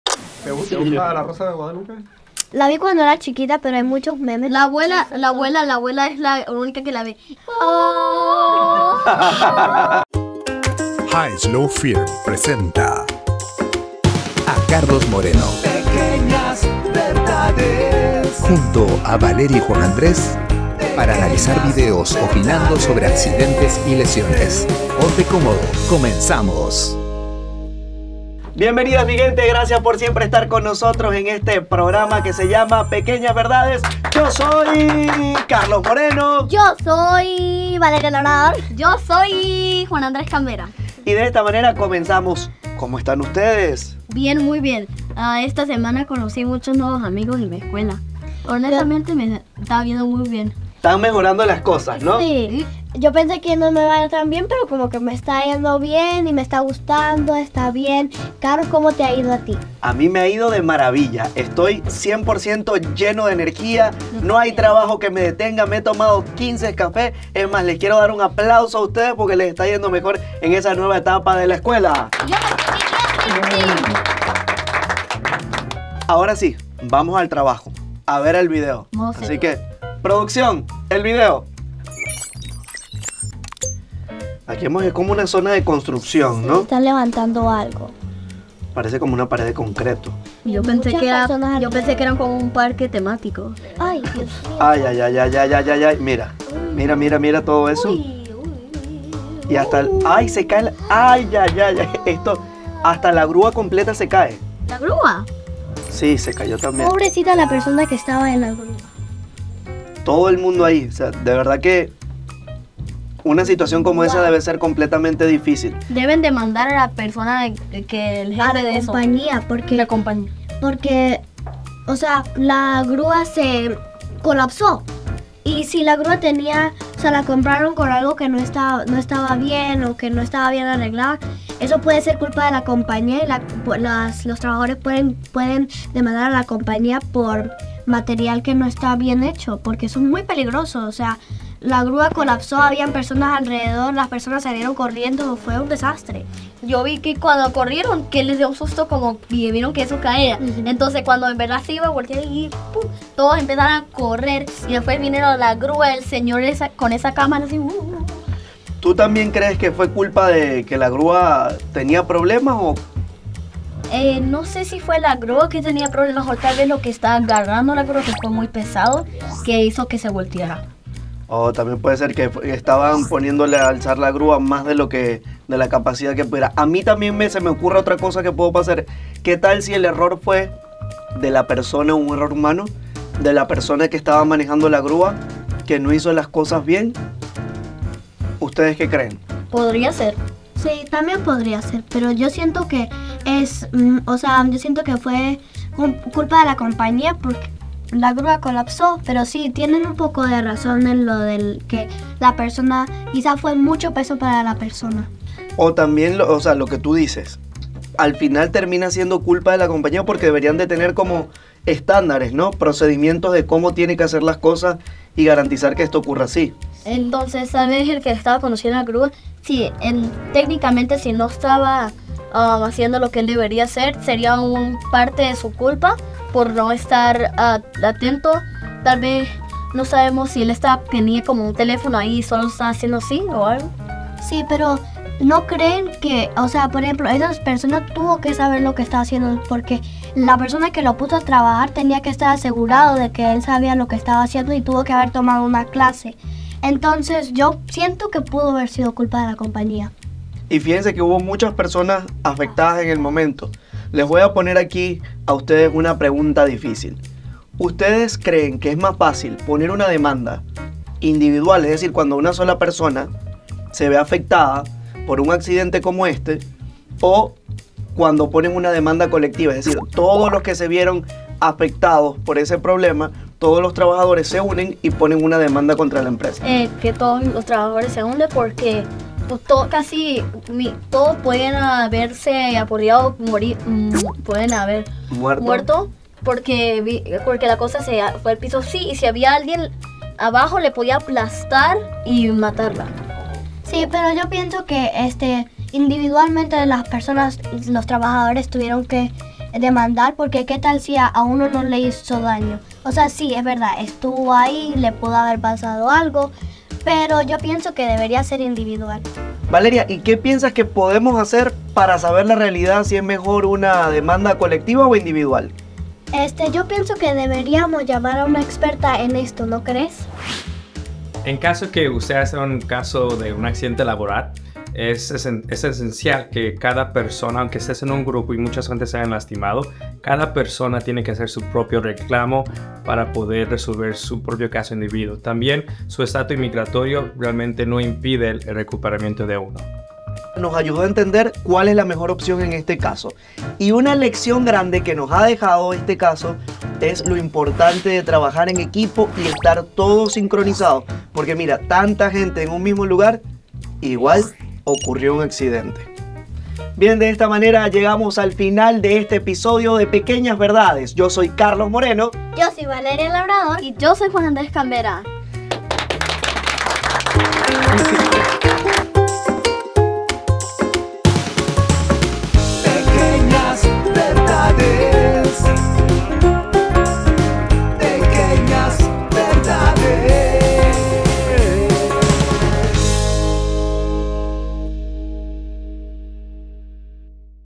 Accidente Laboral Masivo - Ep. 4 2da Temp. Pequeñas Verdades by HLF
👦👧 Accidente Laboral Masivo: ¿Demanda Individual o Colectiva? 🤔

En el nuevo episodio de Pequeñas Verdades, nuestros pequeños nos sorprenden con sus opiniones sobre qué hacer después de un accidente laboral. ¿Es mejor buscar una demanda individual o unirse a otros afectados y hacer una demanda colectiva?

💡 Descubre las divertidas ocurrencias de estos pequeños expertos y sus ideas sobre cómo actuar en estos casos. ¡Quizás nos enseñen algo nuevo!